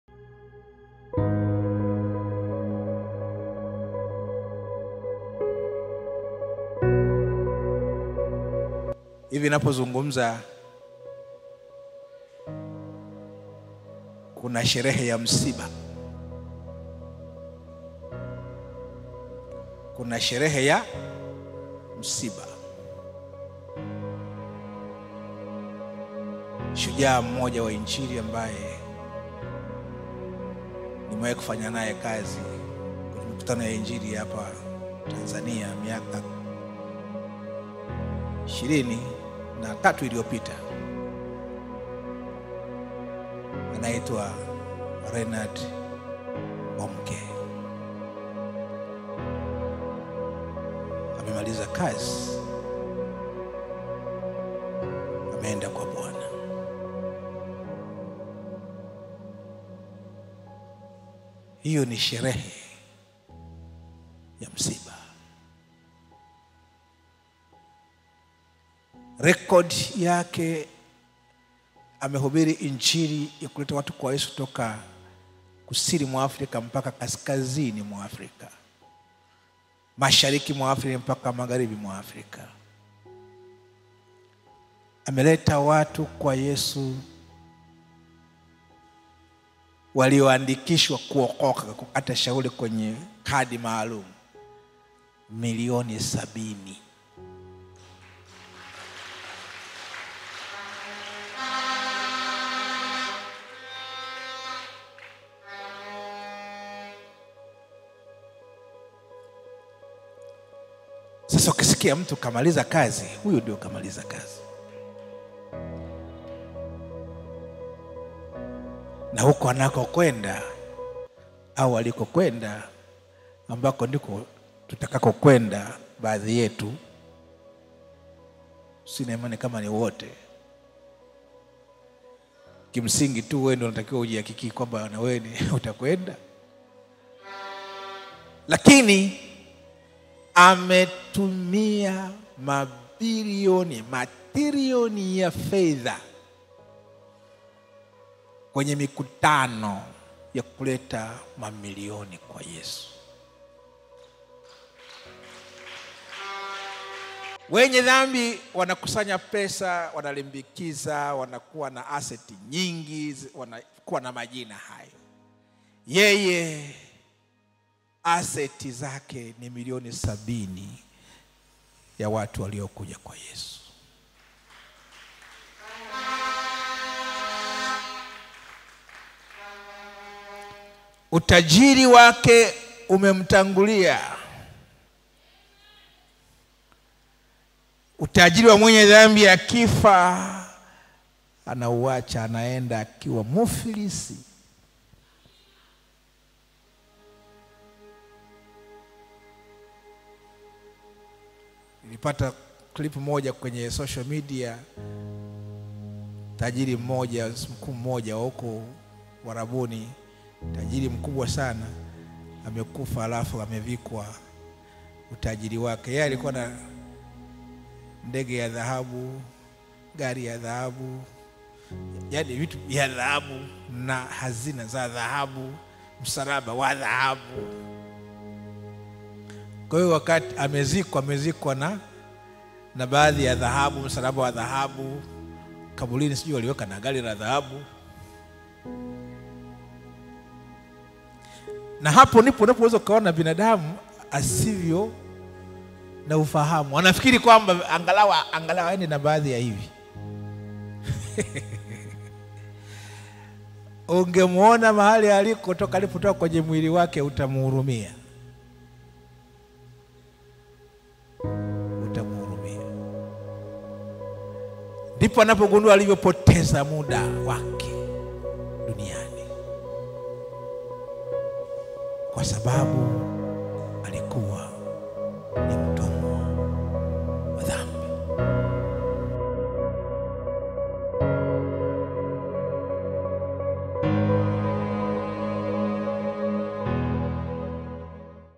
....ALIWEKEZA UTAJIRI WAKE WOTE: BISHOP GAMANYWA AWEKA WAZI MAISHA YA BONNKE!
Ungana na Bishop Gamanywa kupitia Yesu ni zaidi ya maji,mafuta,vitambaa na vitu vyote! ndio maana tunatangaza bali sisi tunamhubiri KRISTO. JUKWAA LA KUMHUBIRI KRISTO HILI HAPA...